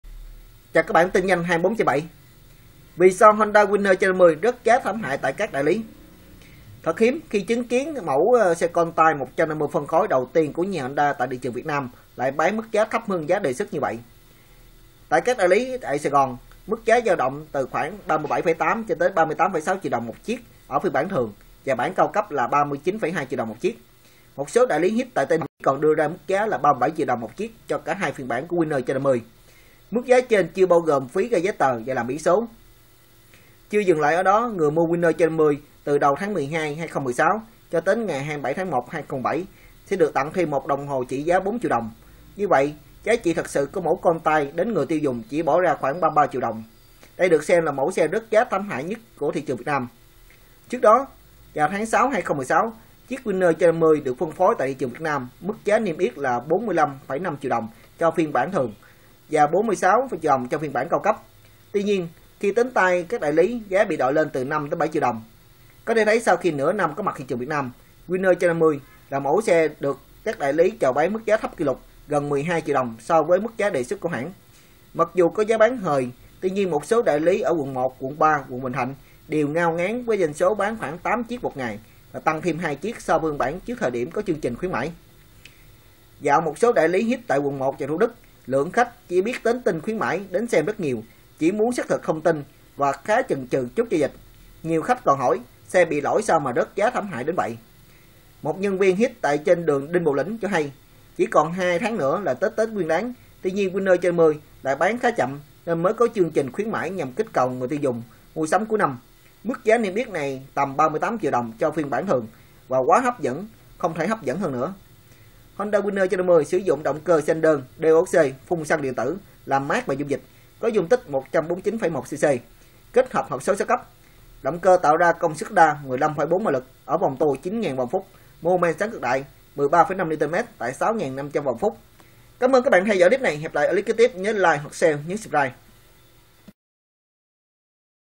Tin nhanh 24/7 - Vì sao Honda Winner 150 rớt giá thảm hại tại các đại lý!???
Những chiếc Honda Winner 150 đang được rao bán với mức giá từ 37 và 38 triệu Đồng cho hai phiên bản và nếu trừ đi phần quà tặng 4 triệu Đồng, xe đang có mức giá bán 33 triệu Đồng thấp hơn 12 triệu Đồng so với mức giá đề xuất của hãng Honda...